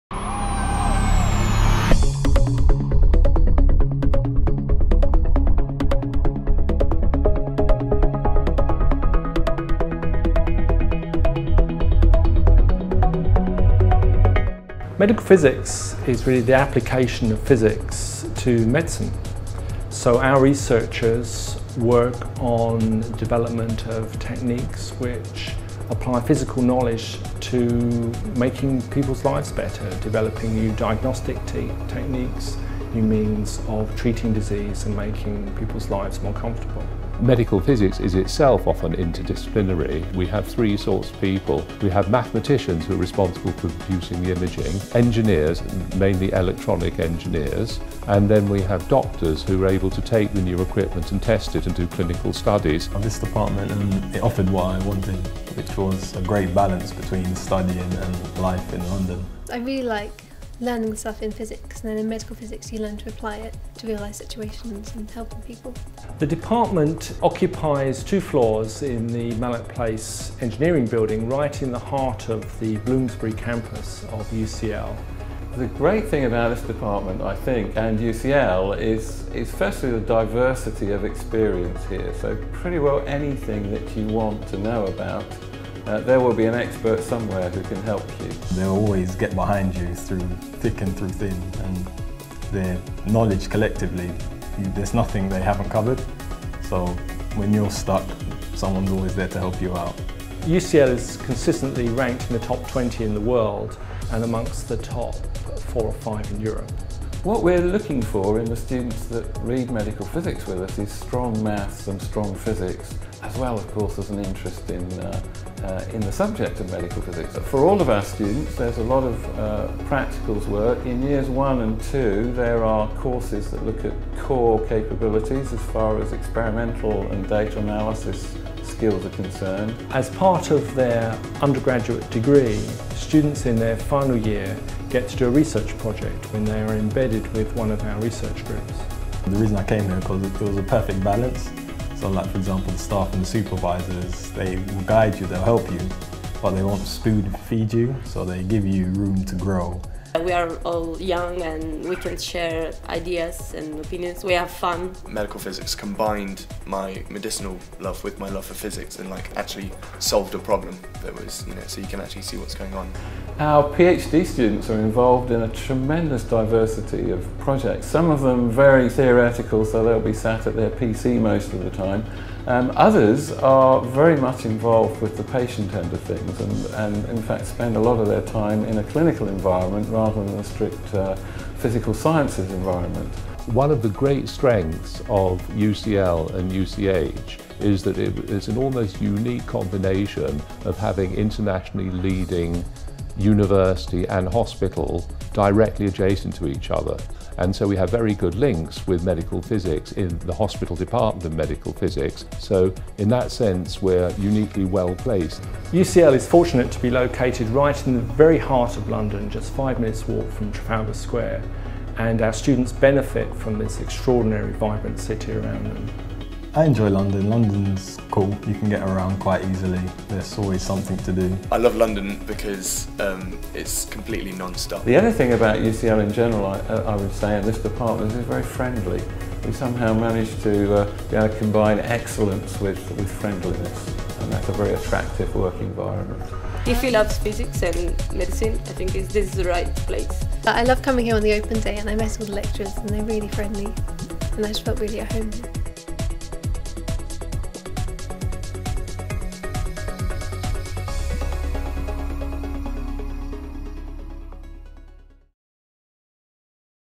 UCL Medikal Fizik ve Biyomedikal Mühendisliği Departmanı
İngiltere'nin önde gelen üniversitesi UCL'de Medikal Fizik ve Biyomedikal Mühendisliği programlarını inceleyin.
Danışmanlık ve başvuru hizmetleri için bizi arayın. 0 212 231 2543